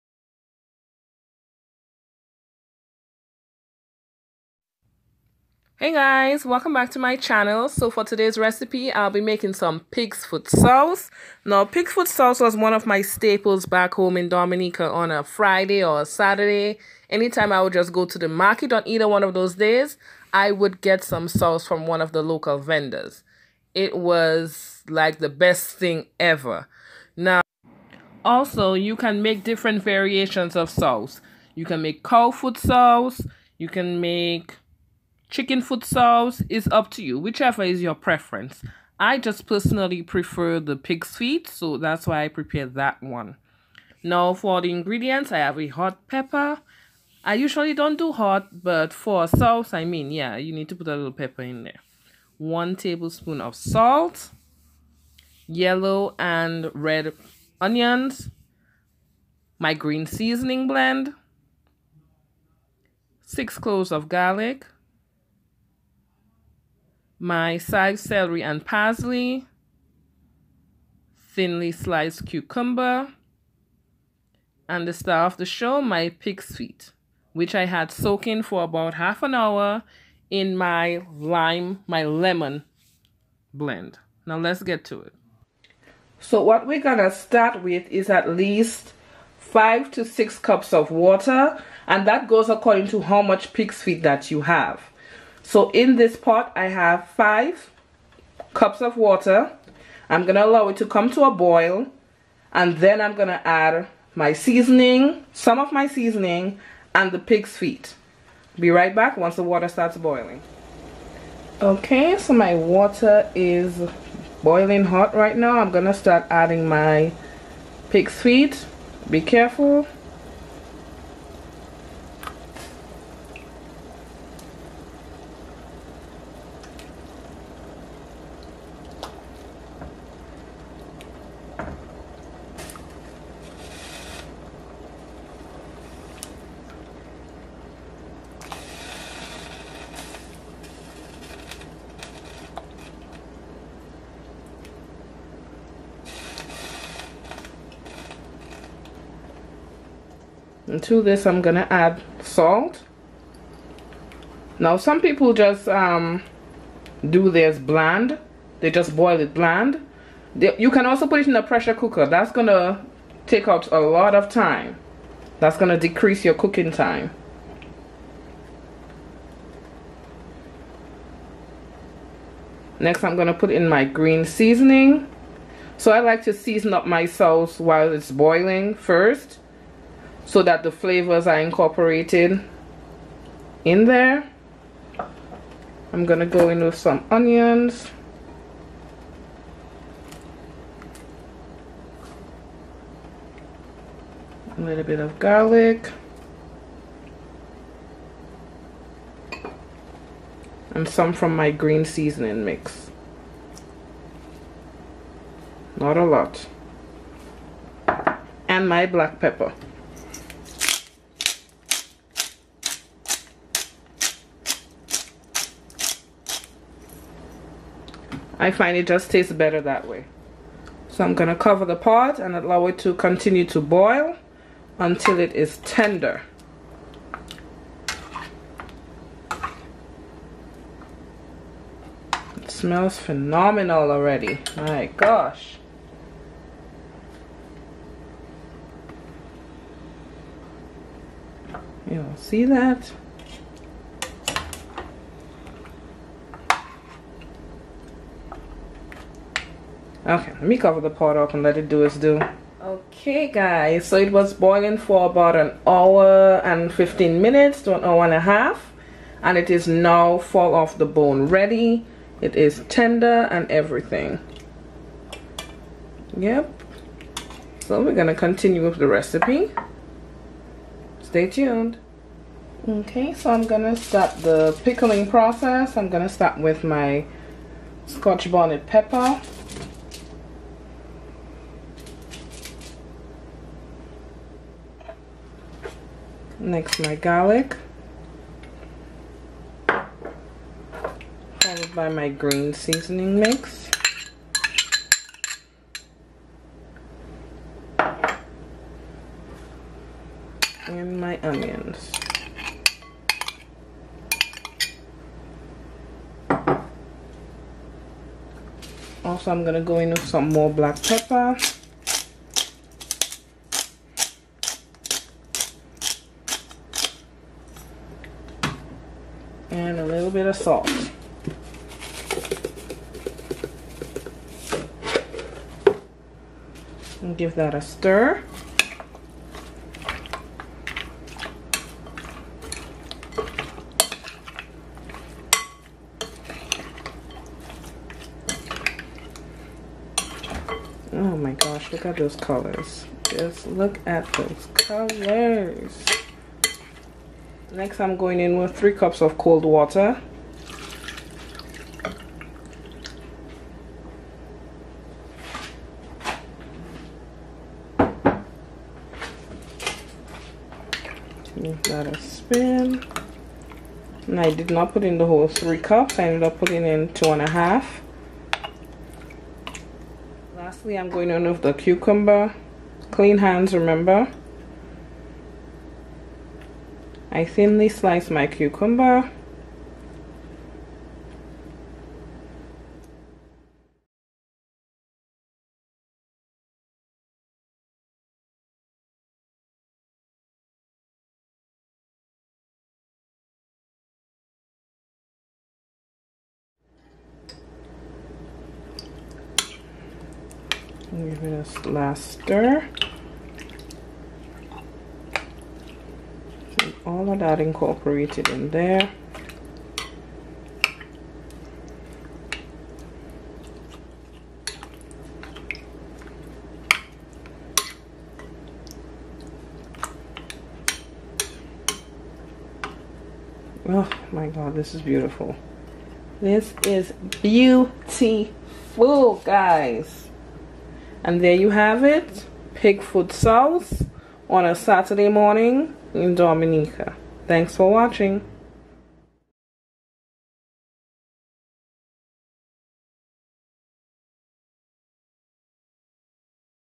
Souse Recipe || FRUGALLYT
It's a beautiful Saturday and I decided what better breakfast is there than pigs foot souse to remind me of my beautiful island Dominica . This video was made with no intention of offending those who do not eat pigs feet for what ever reason. There is also the option of cow foot or chicken feet as substitutes.

Comment if you haven't done so already. Thank you once again to all my subbies both old and new.